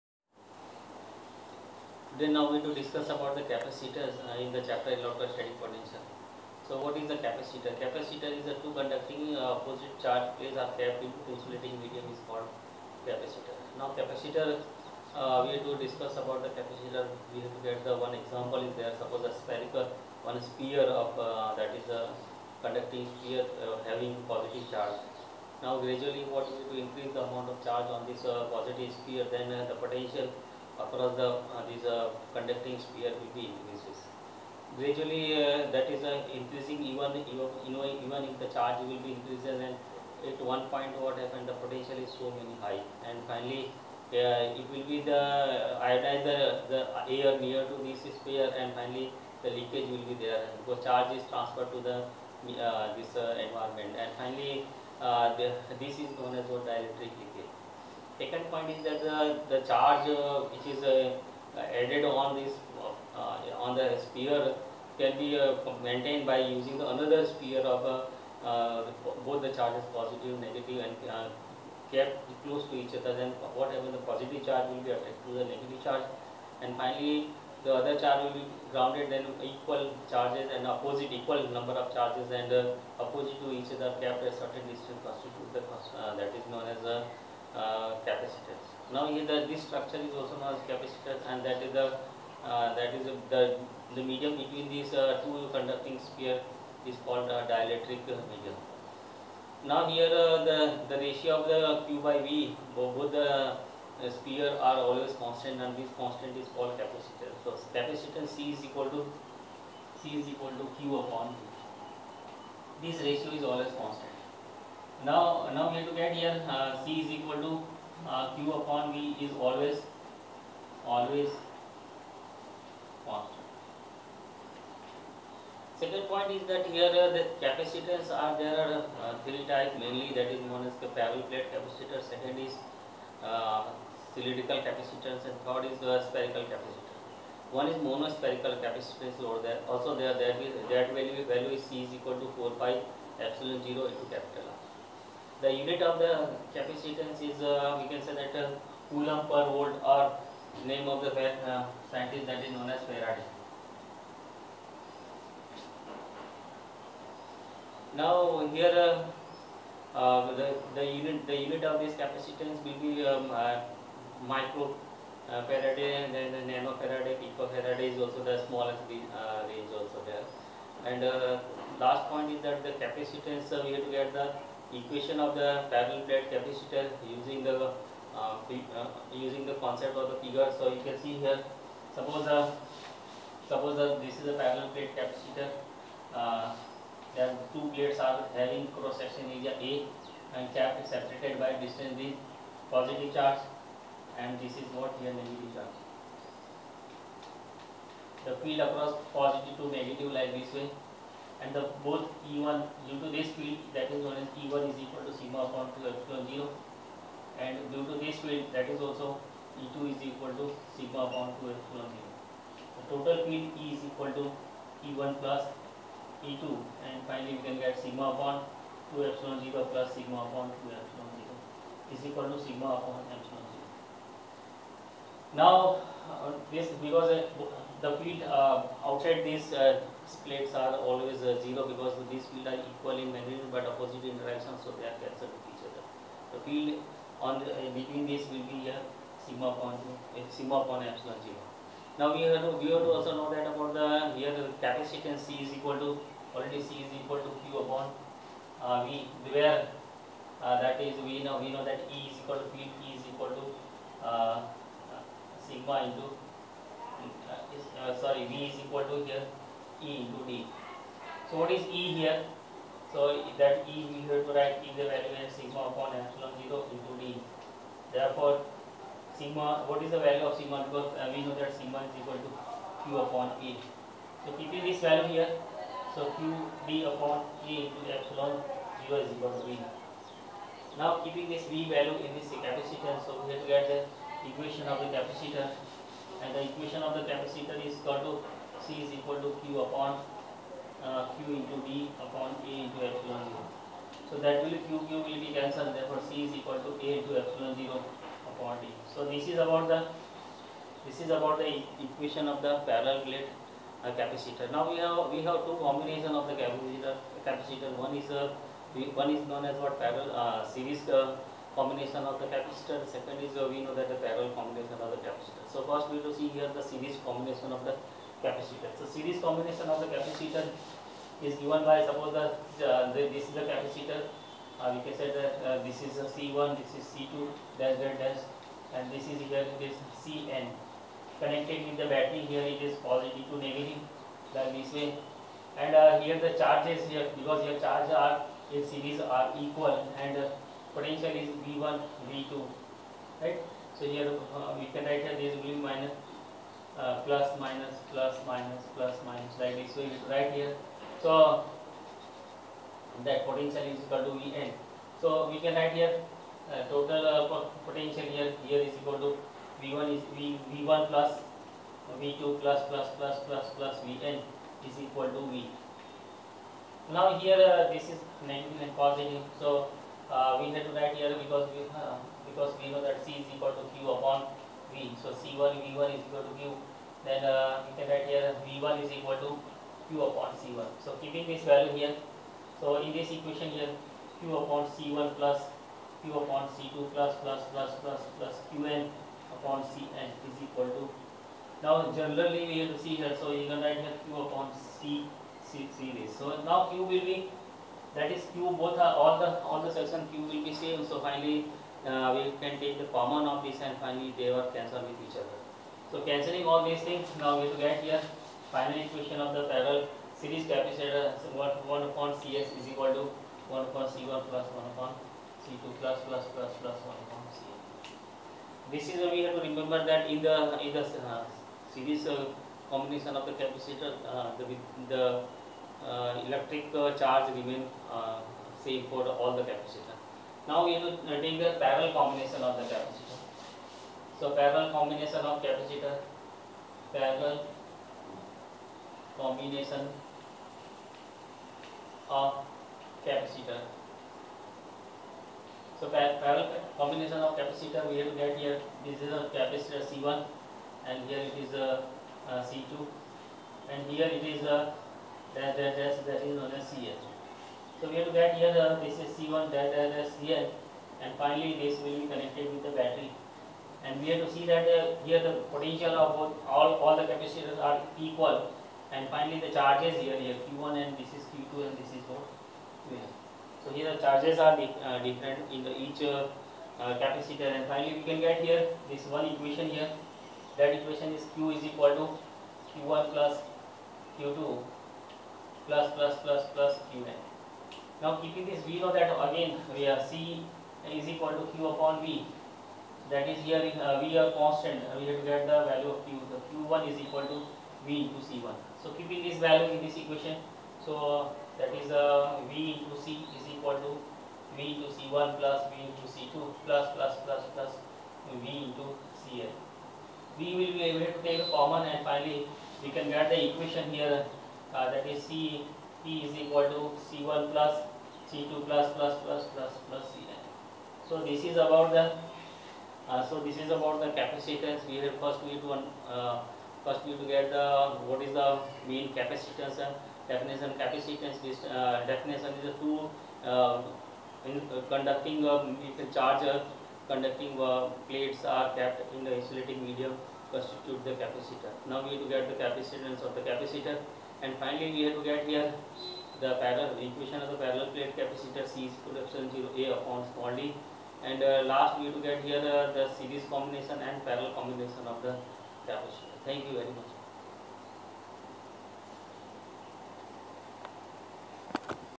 Concepts for combination of capacitor
Here's a description of basic information about capacitor along with concepts related to combination of capacitors.